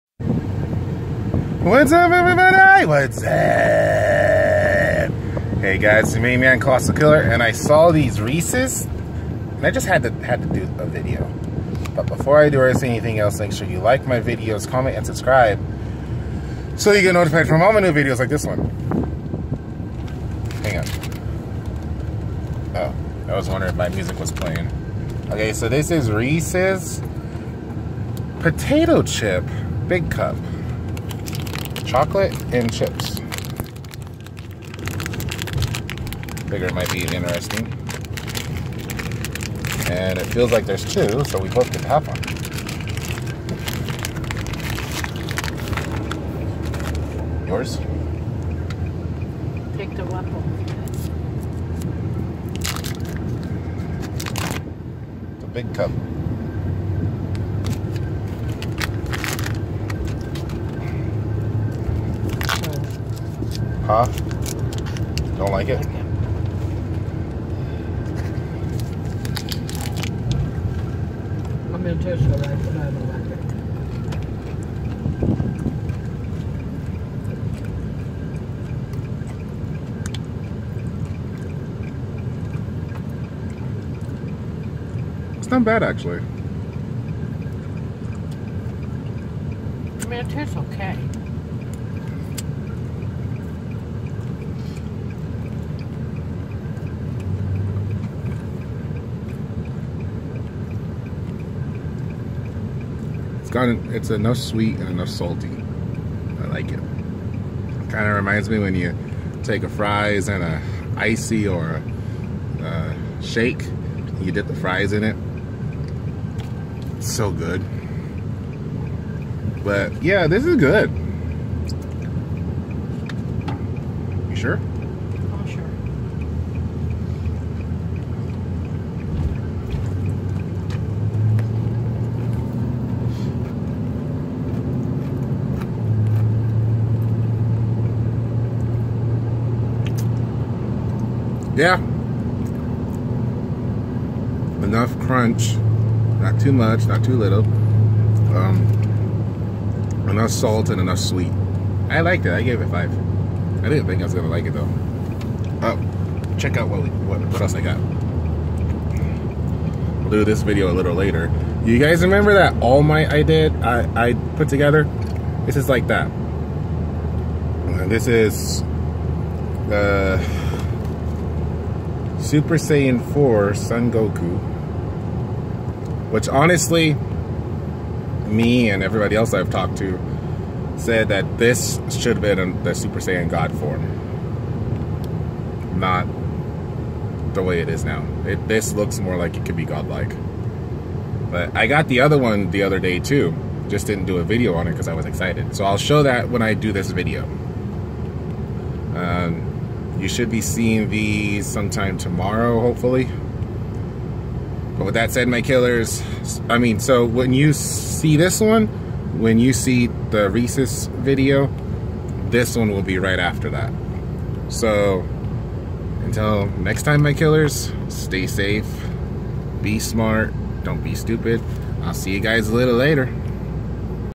review Reese's potato chips big cups
know what it was really good it was not too salty or too sweet it was just right and it had a little crunch Bean chips so with that I gave it five stars I really liked it stay tuned after this video for my Dragon Ball video